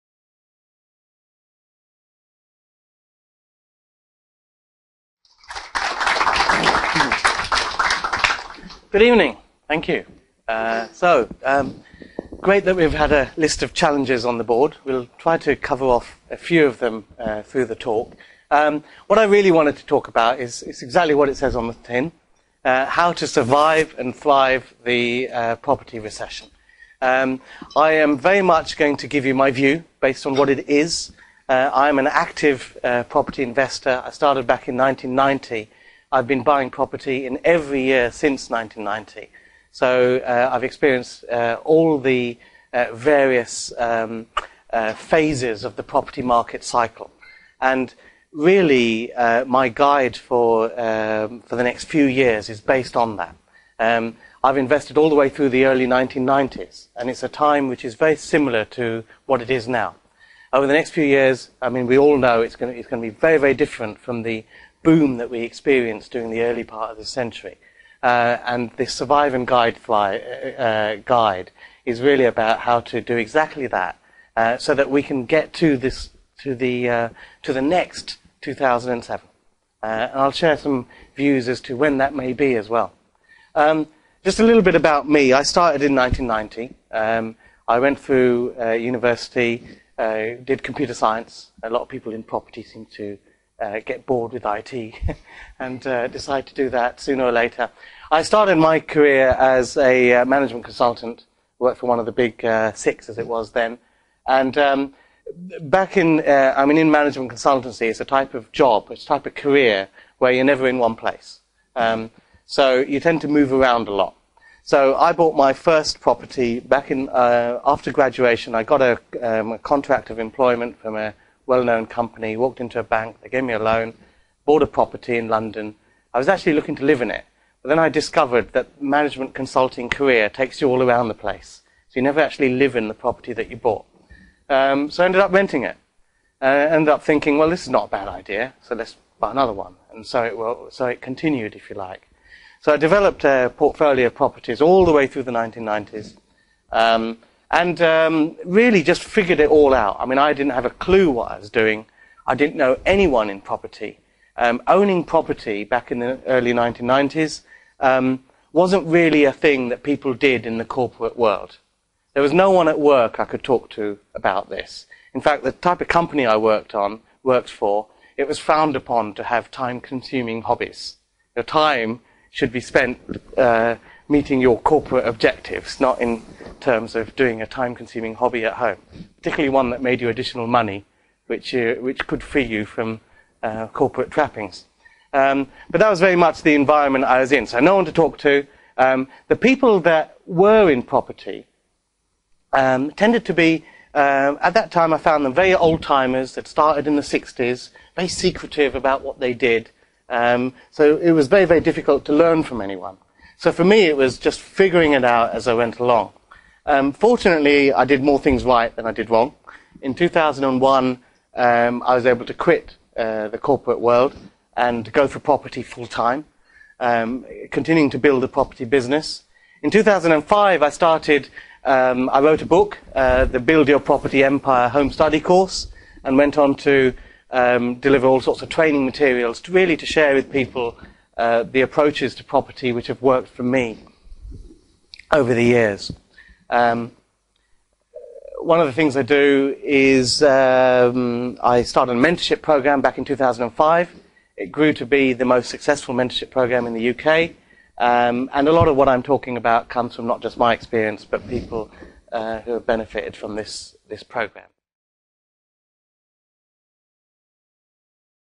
High Flyers Network: London Property Networking Sept 2009 Club features Ranjan Bhattacharya
Ranjan Bhattacharya talks about "your Property Garden" and what strategies and steps you can take now to cultivate your property portfolio in three, four and more years.

Ranjan looks at previous trends and shows how you can study these trends to map out your future in the property investing. A fascinating talk.